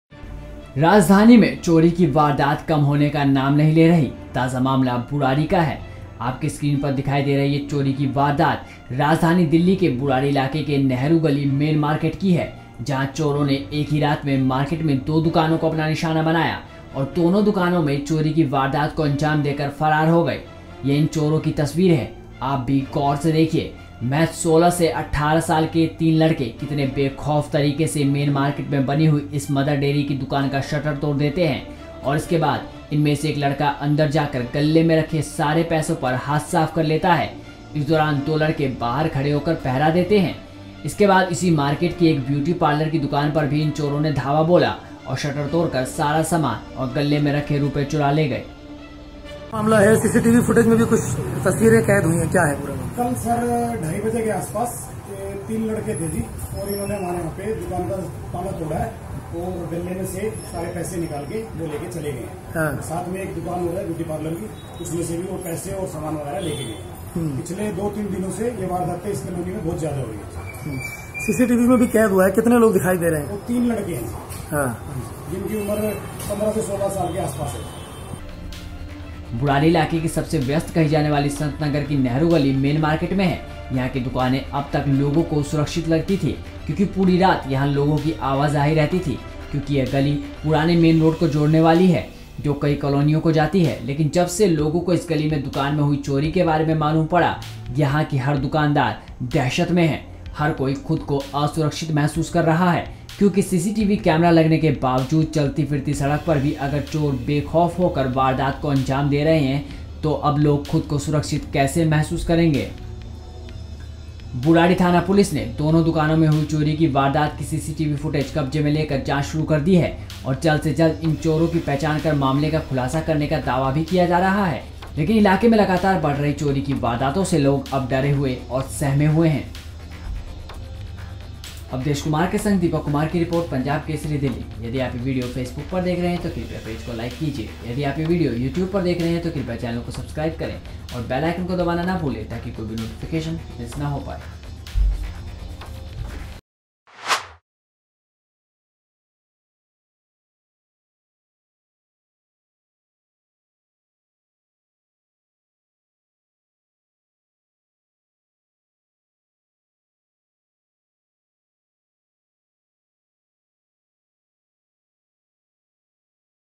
एक ही रात में चोरों ने मुख्य बाज़ार के दो दुकानों को बनाया निशाना | Punjab Kesari Delhi
एक ही रात में चोरों ने मुख्य बाज़ार के दो दुकानों को बनाया निशाना |